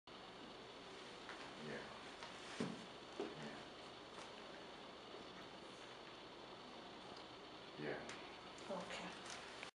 Monofilament test
Demonstration of monofilament test at St Peters Medical Centre in St Kitts